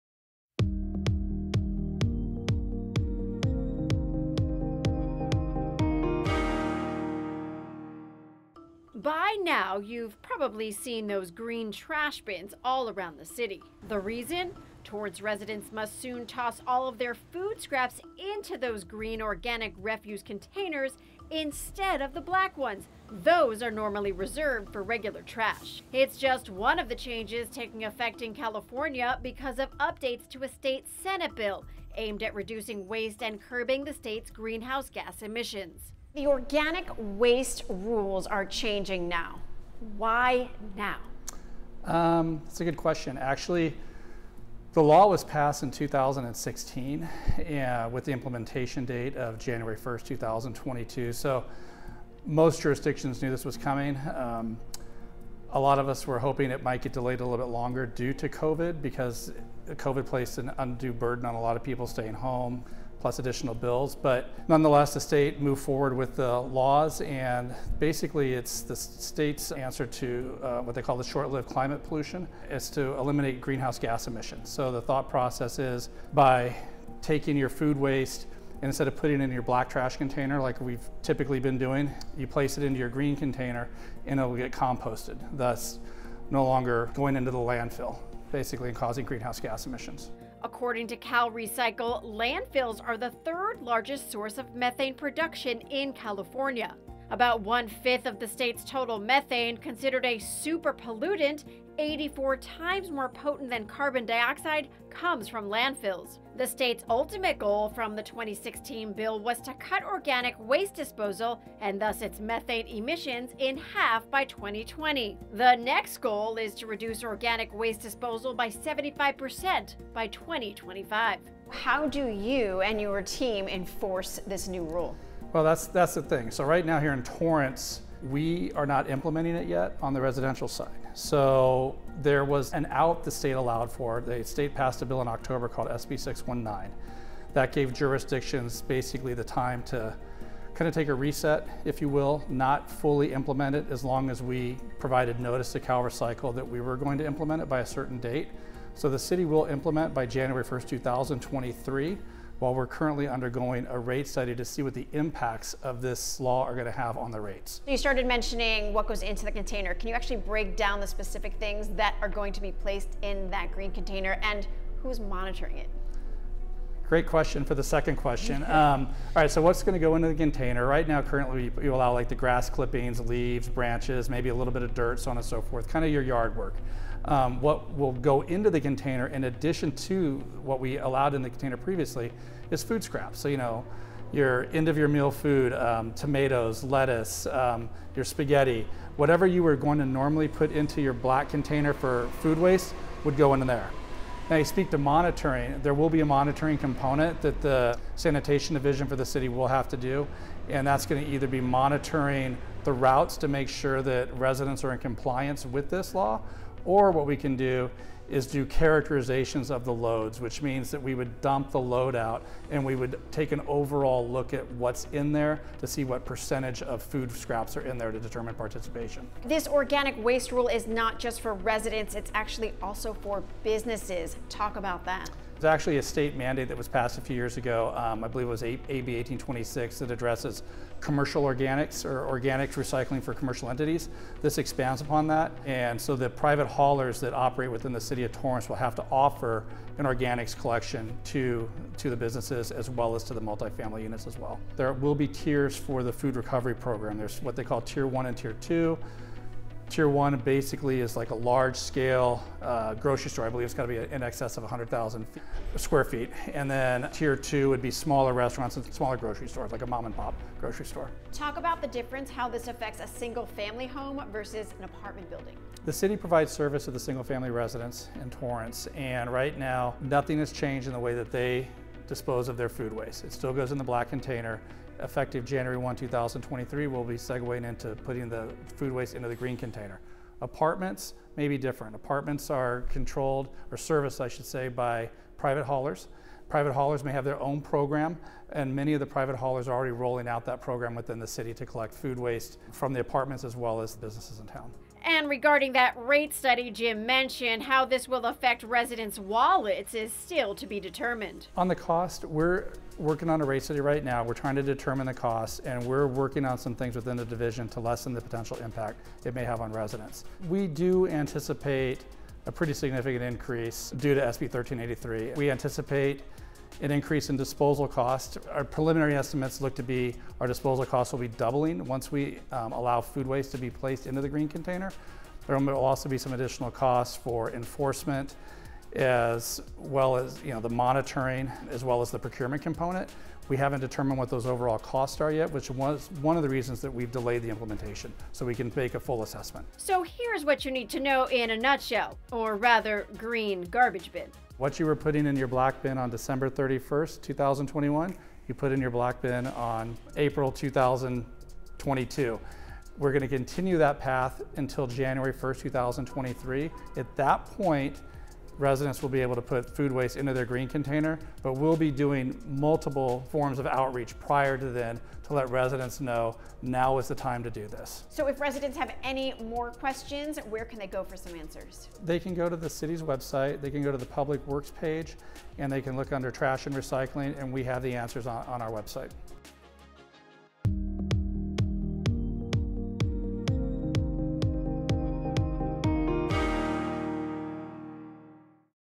Weekends in Torrance - New Organic Waste Rules - City of Torrance, CA
Organic waste rules are changing, and the city highlights what residents need to do.